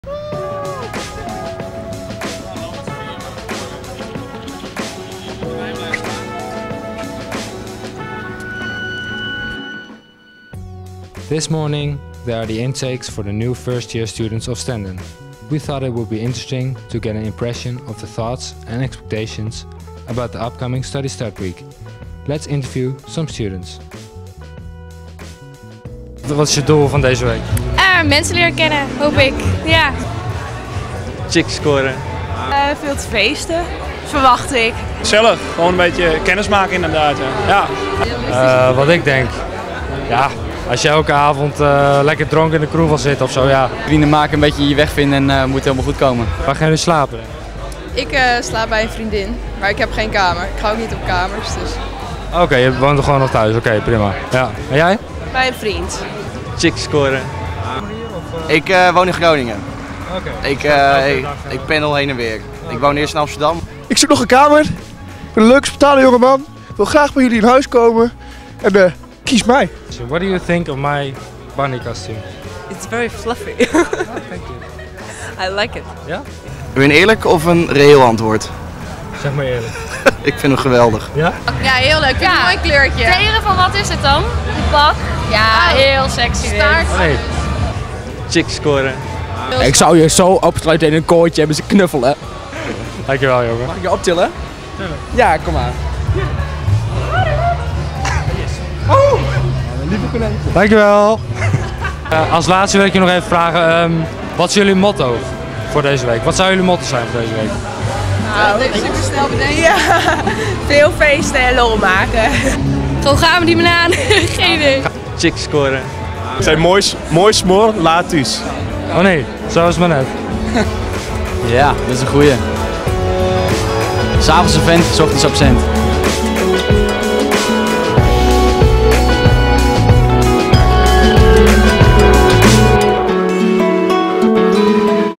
Registration StudyStartWeek - SSW 2013
De Studie start week is een onmisbare week omdat:

• Je krijgt informatie voor de start van je studie.

• Je kunt je laatste dingen met betrekking tot het inschrijven regelen.

• Je leert de weg vinden in de stad Leeuwarden.

• Je maakt kennis met het studentenleven.

• Je maakt kennis met je medestudenten.

• Kortom, het is een week om NIET te missen!